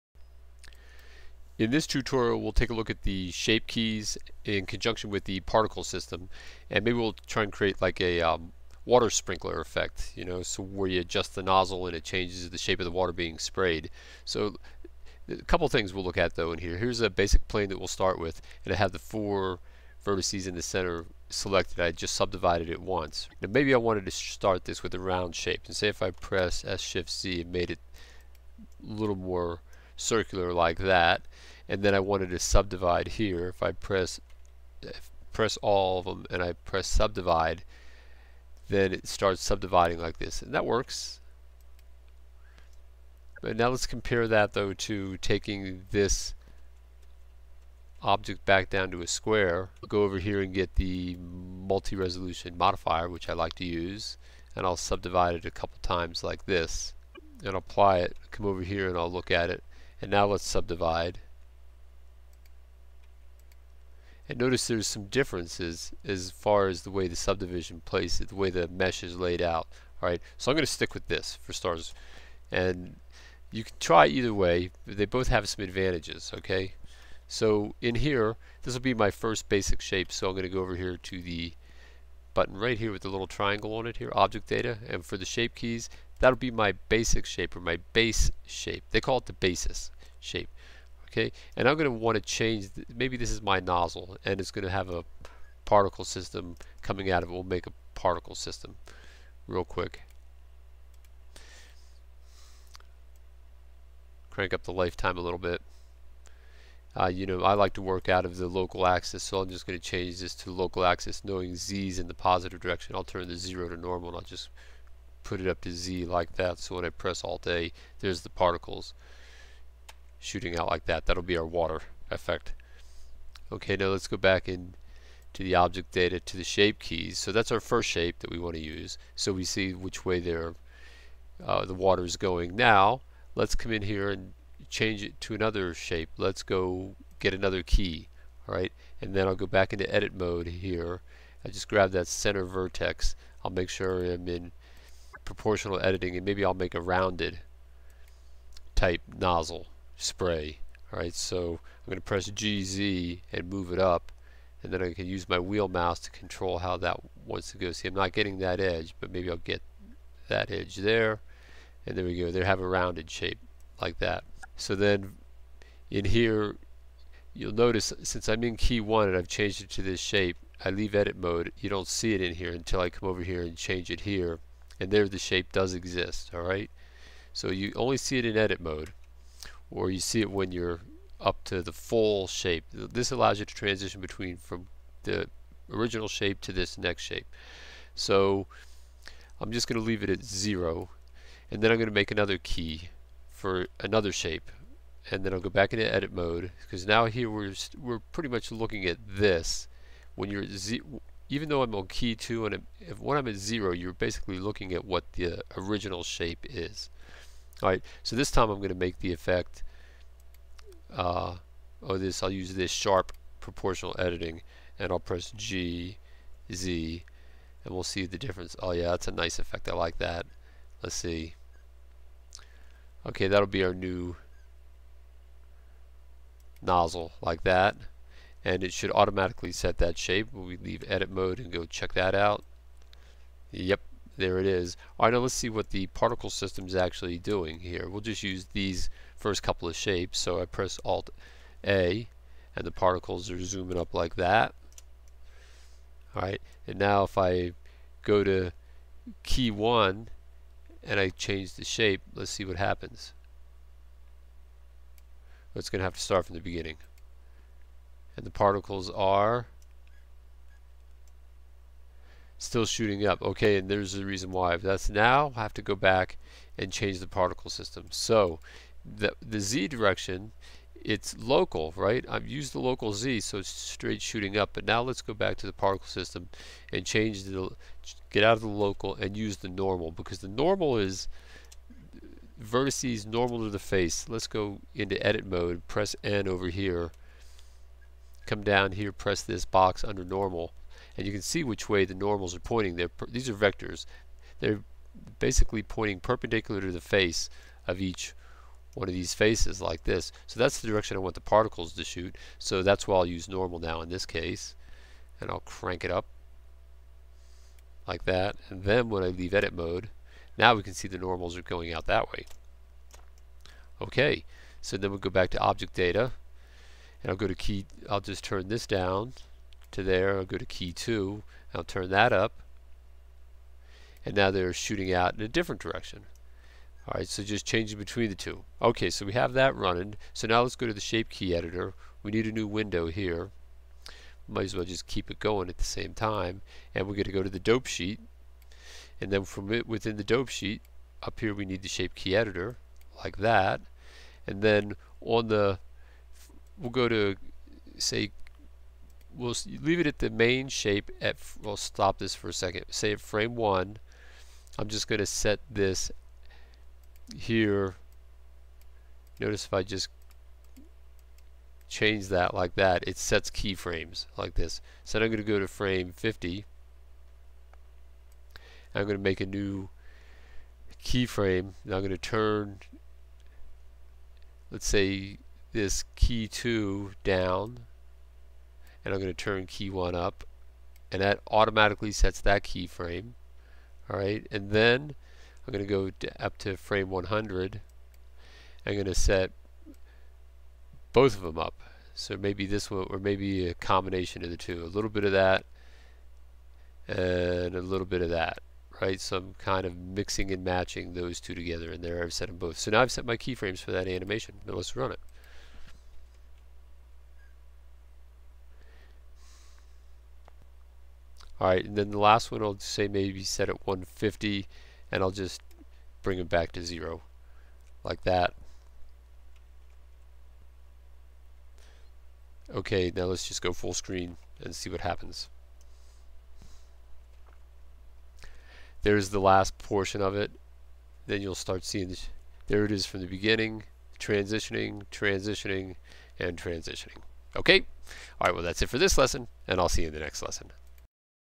Blender Tutorial - Directing Particles with the Shape Keys
In this tutorial for new Blender users, we use the Shape Key Editor to control the shape of our object which then allows us to control the direction of particles that it emits. Blender 2.65 for beginning Blender users.